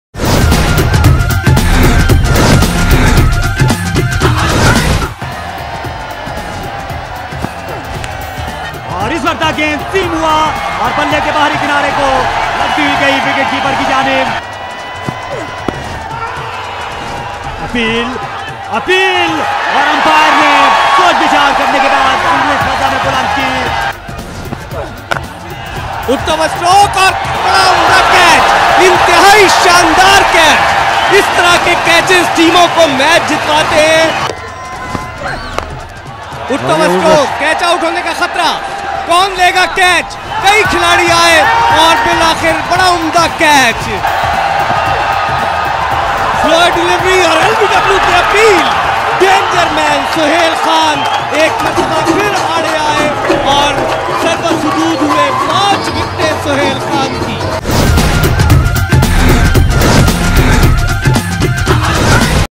Man of the Match - Aftab Qureshi
Aftab Qureshi was the Man of the Match in the final of the Faysal Bank T20 tournament. His amazing batting and bowling performance earned him the title.